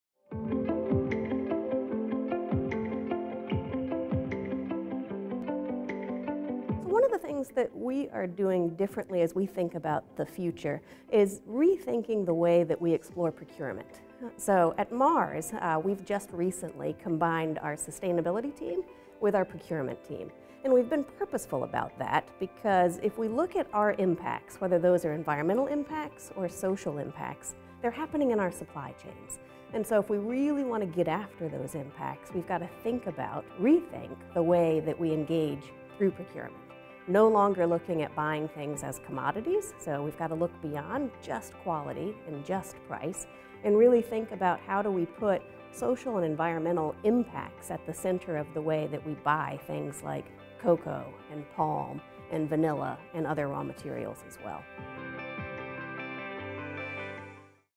The Decisive Decade: Mars | Impact
At the BSR19 Conference, we spoke to a range of BSR Members about their sustainability achievements during the 2010s and their ambitions for the coming decade.